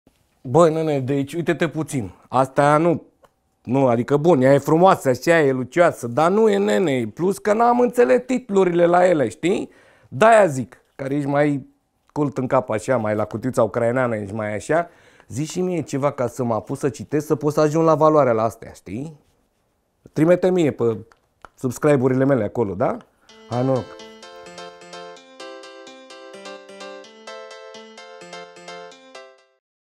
Dă ce să nuse citește dăcâ cotoru la carte! episodoi
În carte e toate litrelele care trebe!!!☝️💪🤙

Bine ai venit în lumea lui Dorin Zidaru'.
Mare atentie e DorinZidaru' nu Dorel ! E o mică mare diferență. Deci nu Dorel , da?!
Satiră de Marius Florea Vizante (Teatrul de Comedie, Grupul Voua, Liber ca pasarea cerului, Filantropica, Restul e tacere  )